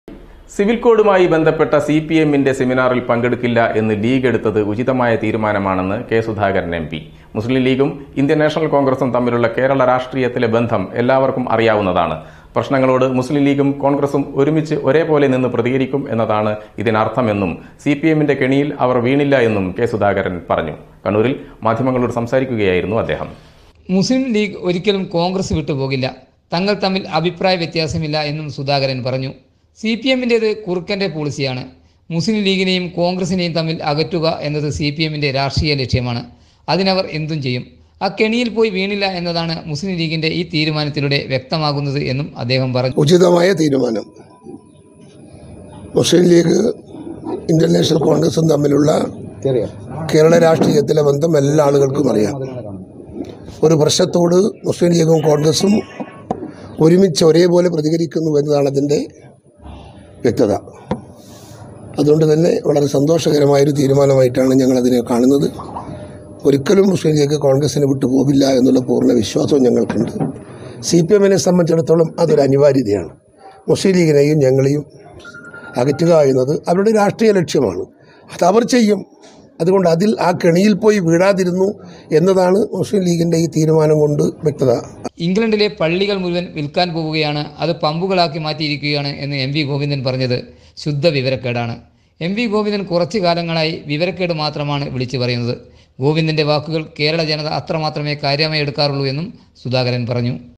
സിവിൽകോഡു CPM സെമിനാർ പങ്കെടുക്കില്ല ലീഗ് എടുത്തത് ഉചിതമായ തീരുമാനമാണെന്ന് കെ സുധാകരൻ
സിവിൽകോഡുമായി ബന്ധപ്പെട്ട സിപിഎമ്മിന്റെ സെമിനാറിൽ പങ്കെടുക്കില്ല എന്ന് ലീഗ് എടുത്തത് ഉചിതമായ തീരുമാനമാണെന്ന് കെ സുധാകരൻ